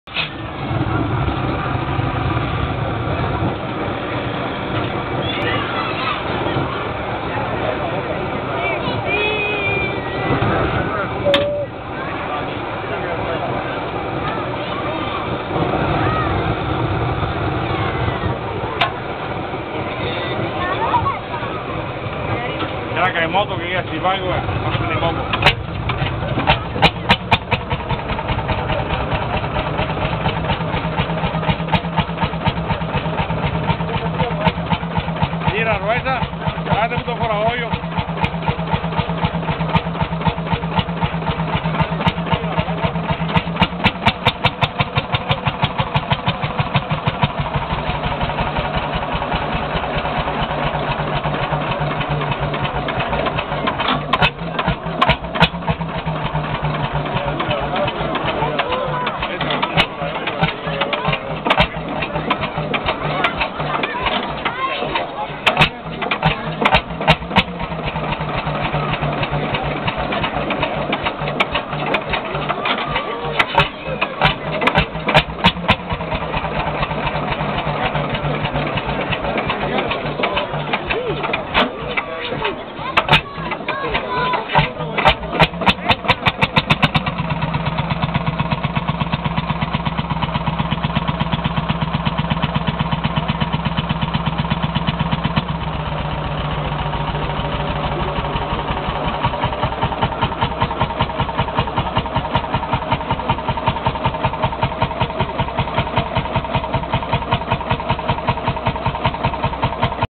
Landini testa calda Settembre 2012
Avviamento di trattore Landini a testa calda  9.000 cc di cilindrata monocilindrico del 1954.